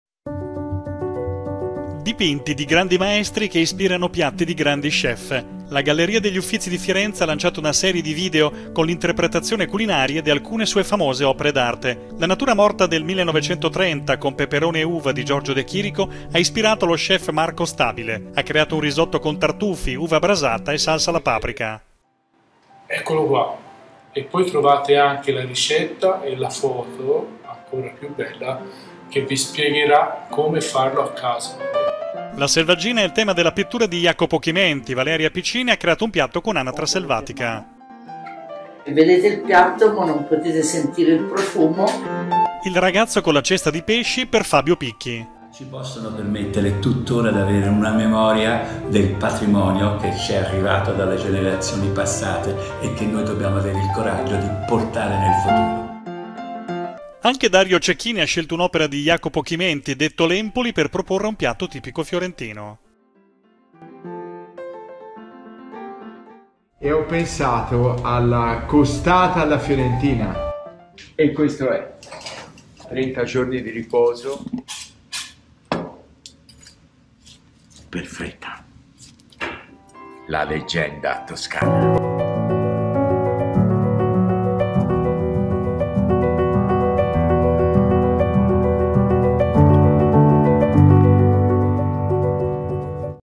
Arte e cibo: le Gallerie degli Uffizi di Firenze presentano piatti ispirati alle opere d'arte
Con i musei aperti ad intermittenza, le Gallerie degli Uffizi di Firenze presentano un'originale abbinamento: chef stellati che preparano piatti ispirati alle opere d'arte presenti nel museo. Uno show cooking on-line che sta avendo molto successo